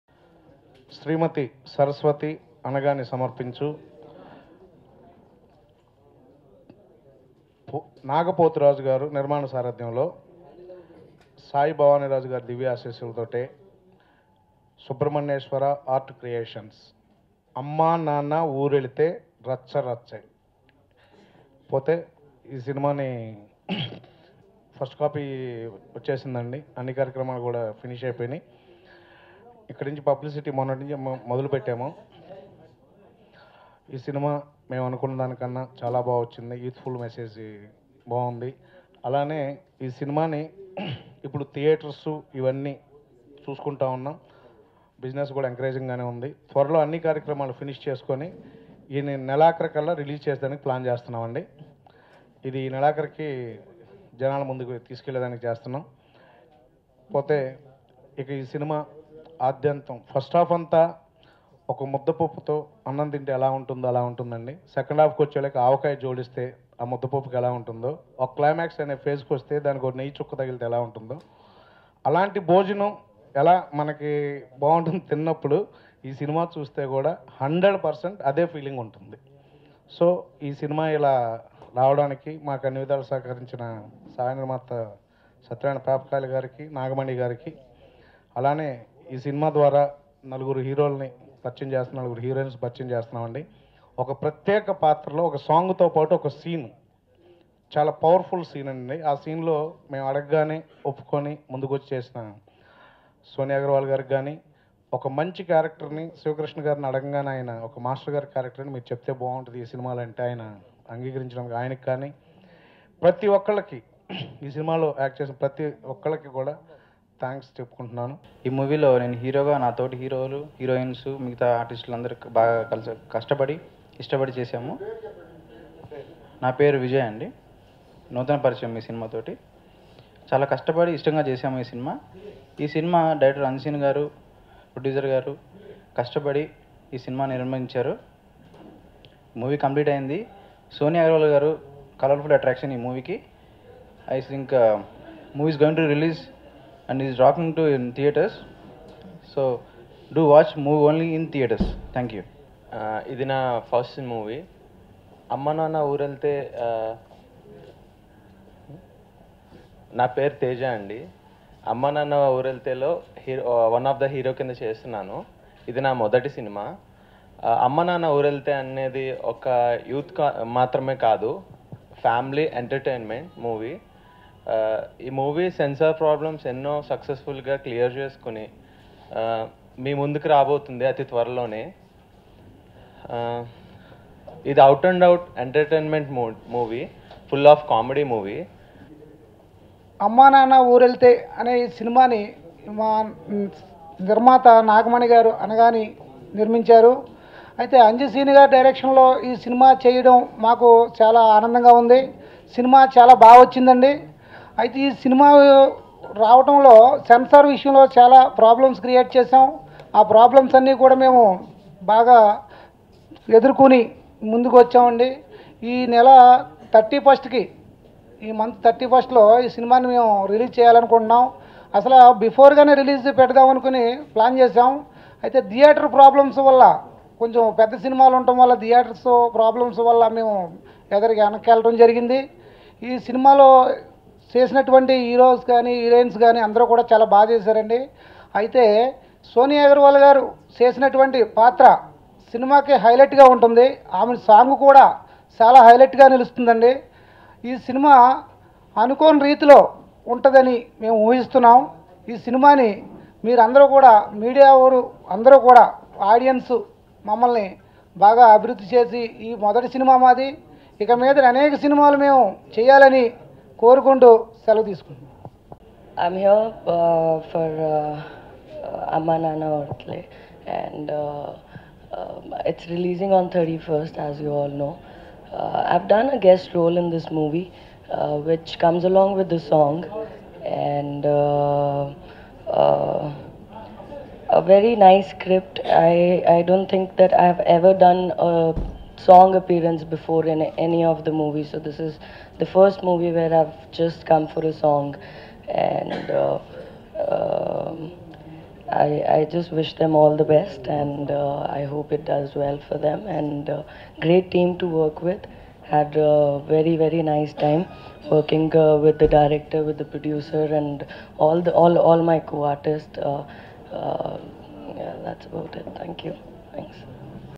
Amma Nanna Oorelithe Press Meet Video
Watch Amma Nanna Oorelithe Movie Press Meet Video,

Casting: Tanusha Ashwi Sushmita Silpa's Amma Nanna Oorelithe Movie Press Meet